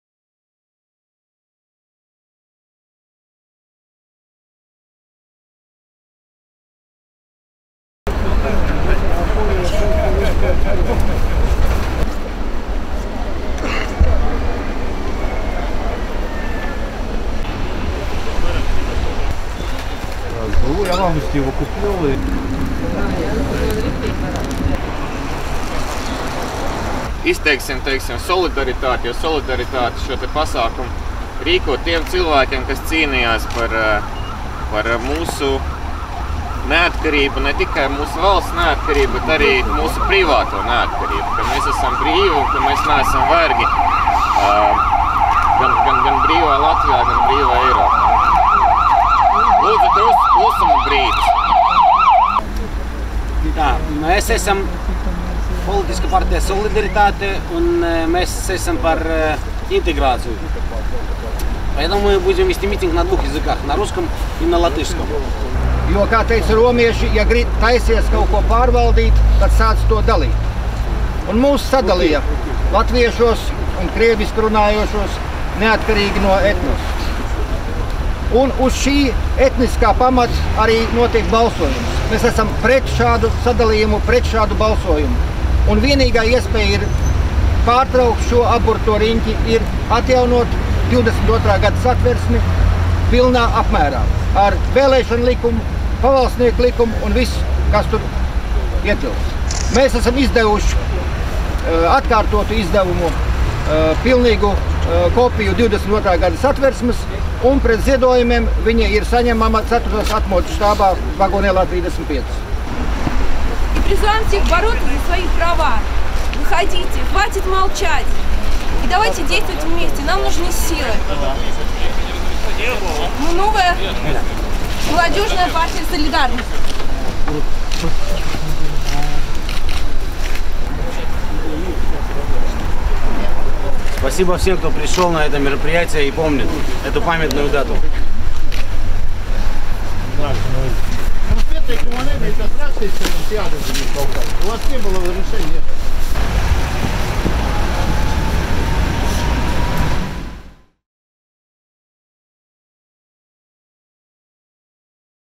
mītiņš pie 1905 gada pieminekļa
Politiskā partija "Solidaritāte" organizēja mītiņu -piemiņas pasākumu 11. novembra krastmalā pie 1905.gada cīnītāju pieminekļa ar devīzi : "Varu Darbam nevis Kapitālam" un "Neviens nav aizmirsts, Nekas nav aizmirsts".